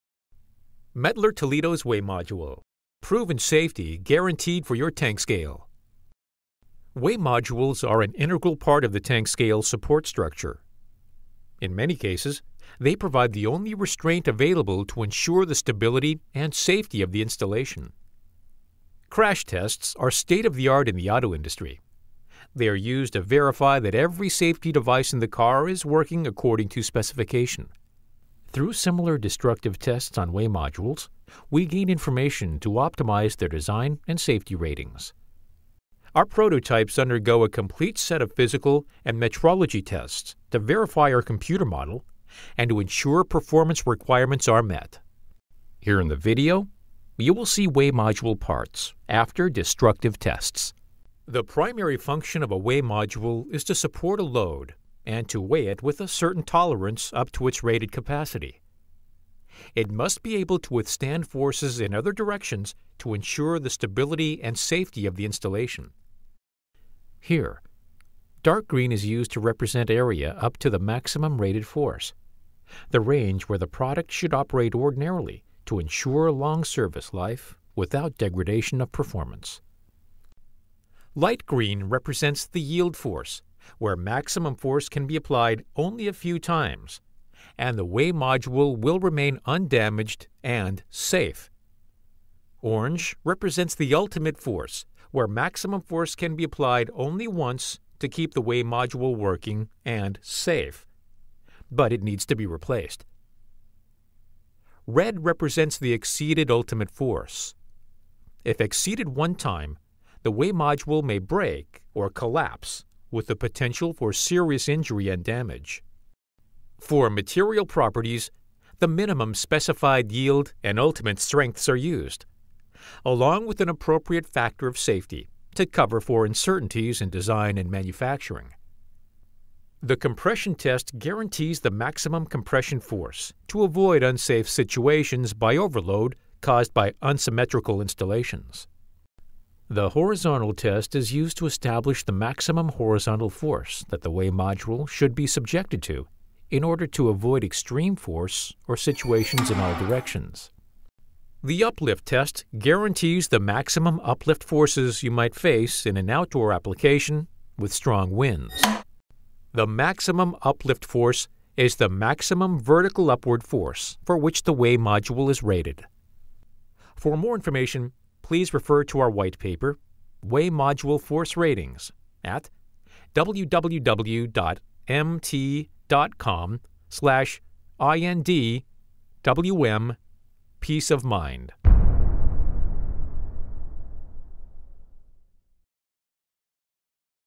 METTLER TOLEDO Weigh Module Proven Safety - METTLER TOLEDO Industrial - en
Weigh modules support scale structures that are often subjected to extraneous forces in various directions. In addition to supporting and weighing a load, weigh modules must be able to resist extraneous forces to ensure the stability and safety of the installation. For this reason METTLER TOLEDO provides comprehensive datasheets for its weigh modules that list rated, yield, and ultimate force ratings for the various directions in which its weigh modules may be loaded. This provides the scale designer with all the information necessary to make an informed choice in selecting a weigh module for a given situation. These ratings are established during a rigorous design and verification process, which includes testing physical samples to destruction.
Crash tests are state-of-the-art in the auto industry. They are used to verify that every safety device in the car is working according to specification.  Through similar destructive tests, we gain information  to achieve a safe product.